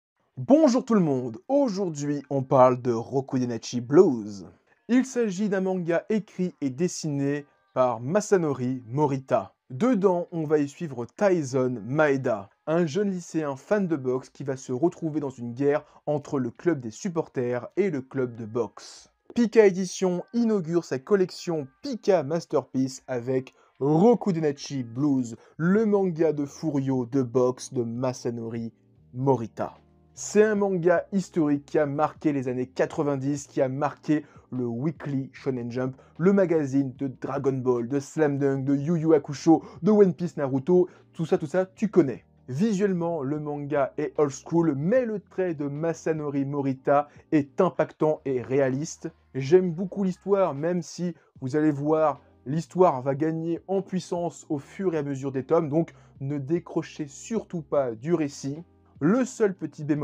Mini MANGA À CHAUD #15 : ROKUDENASHI BLUES
Aujourd'hui bagarre et boxe avec le Furyo Rokudenashi Blues !